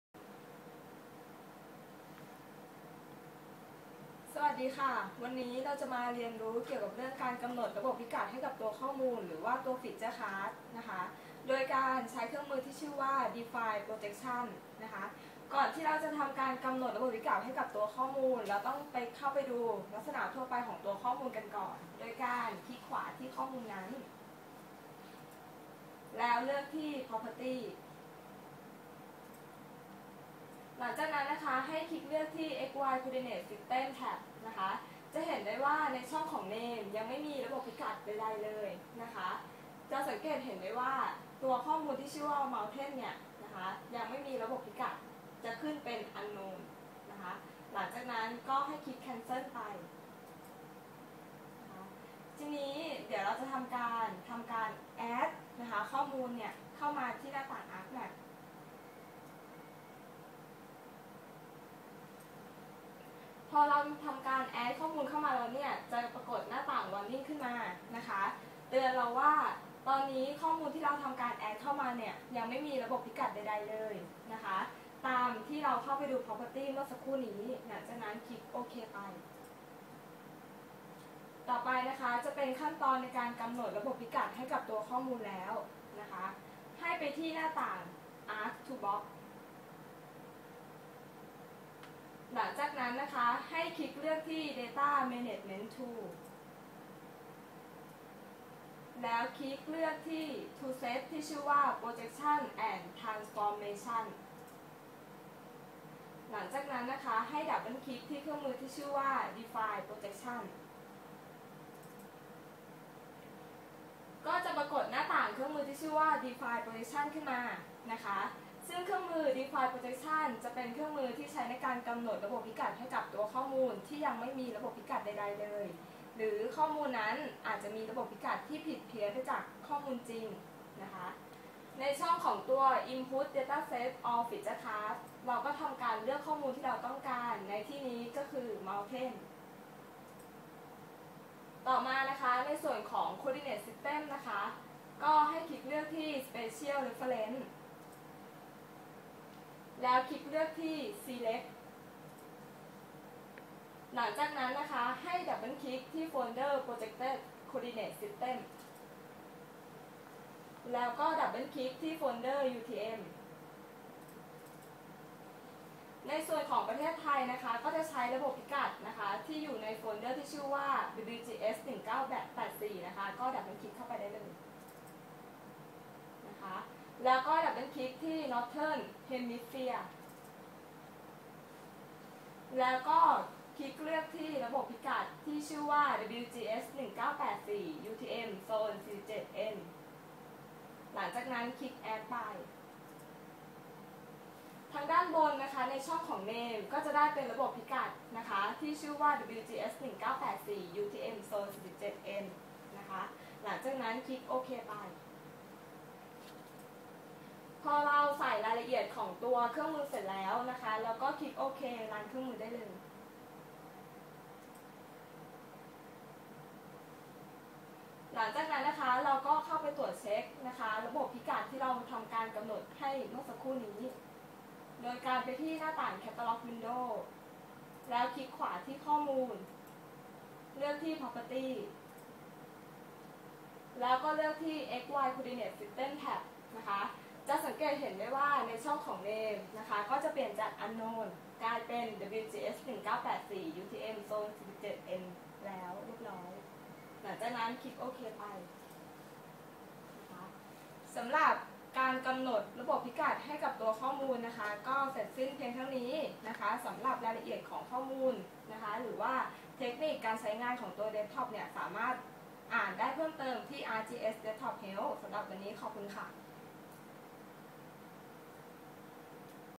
การใช้ Define Projection
อธิบายการใช้ Define Projection ซึ่งเป็นการกำหนดระบบพิกัด ไม่เหมือนกับการใช้ Project ที่เป็นการแปลงระบบพิกัดนะครับ